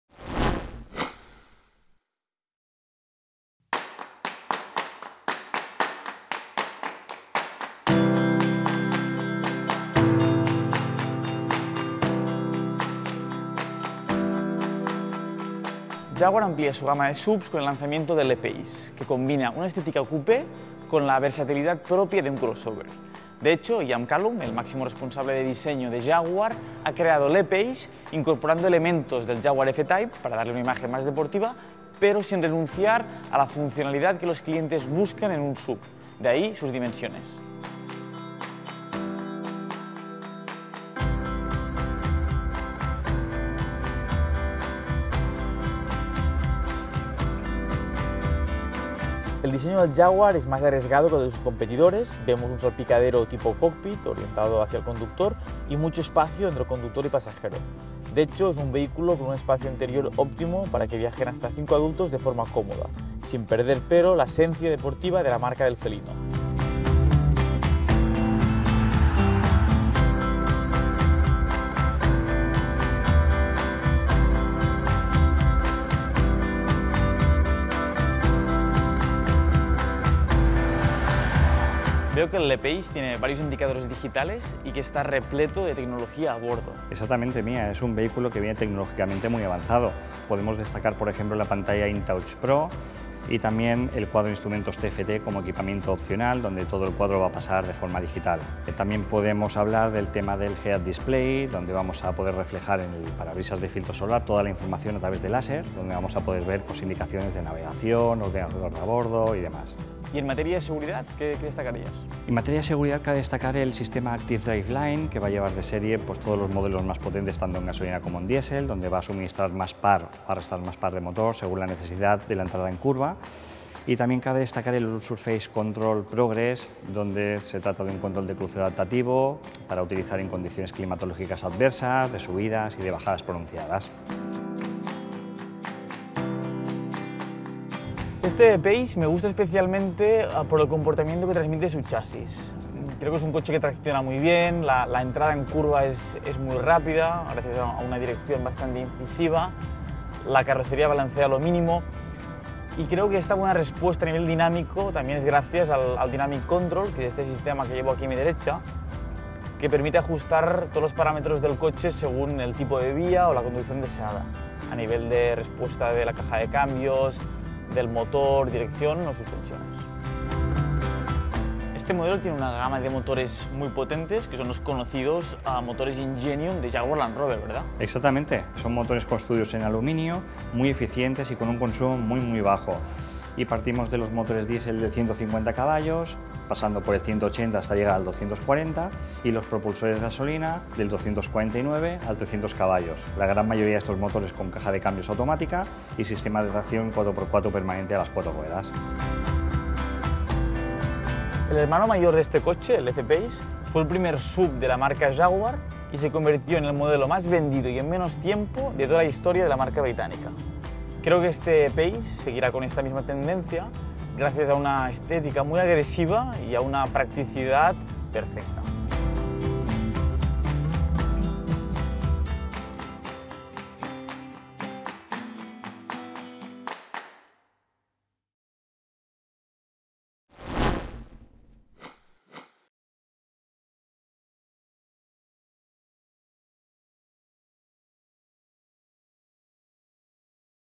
Jaguar E-PACE | Prueba / Test / video en español | quadis.es
Jaguar amplía su gama de SUVs con el lanzamiento del E-Pace, que combina una estética cupé y deportiva con la versatilidad y funcionalidad propia de un crossover.

Ian Callum, máximo responsable del diseño de Jaguar, ha diseñado el E-PACE incorporando elementos del Jaguar F-Type para darle una imagen más deportiva, tal y como se espera en un modelo de la marca del felino. Pero por otro lado, ha querido obtener la funcionalidad que también buscan los clientes en un SUV, y de ahí que las dimensiones fuesen acordes con este objetivo. En el E-Pace pueden viajar hasta cinco adultos de forma cómoda pero sin perder la esencia deportiva de la marca Jaguar.
En el interior Jaguar ha arriesgado un poco más que sus competidores, podemos ver un salpicadero tipo cockpit, orientado hacia conductor y con un espacio claramente delimitado entre conductor y pasajero. Además los detalles, los materiales y los acabados son de muchísima calidad. Y acompañan a 2 pantallas; una pantalla interactiva del conductor de 12,3 pulgadas y otra pantalla Touch Pro de 10”, con unos gráficos excelentes y una usabilidad muy trabajada, de hecho es como navegar con el propio Smartphone.

Los motores Ingenium son construidos en aluminio y con tecnología que los hace muy eficientes y de consumo muy bajo. Los diésel se venden con potencias de 150, 180 y 240 CV, mientras que los de gasolina se ofrecen en 249 y 300 CV. La mayoría de versiones montan cambio automático de nueve velocidades y tracción integral permanente.

El F-PACE fue el primer SUV de la historia de Jaguar y se convirtió en el modelo que más se vendió y en menos tiempo en toda la historia de la marca británica. Y este nuevo E-PACE seguirá con esta tendencia gracias a la combinación de una estética agresiva con una practicidad perfecta.